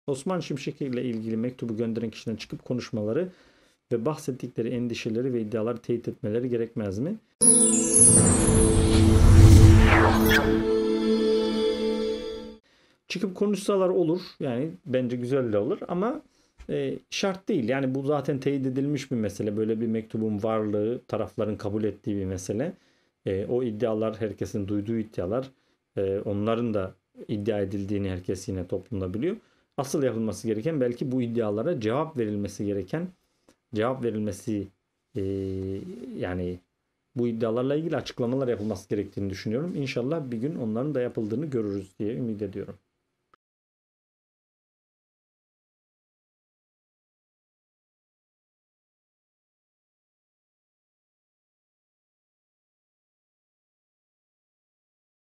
Osman Şimşek’e ilgili mektubu gönderen kişiler, neden konuşmuyor?
İzleyici Sorusu: "Osman Şimşek’e ilgili mektubu gönderen kişilerin çıkıp konuşmaları ve bahsettikleri endişeleri ve iddiaları teyit etmeleri gerekmez mi?"

Kanalımıza madde destek vererek çalışmalarımızın devam etmesinine katkıda bulunabilirsiniz.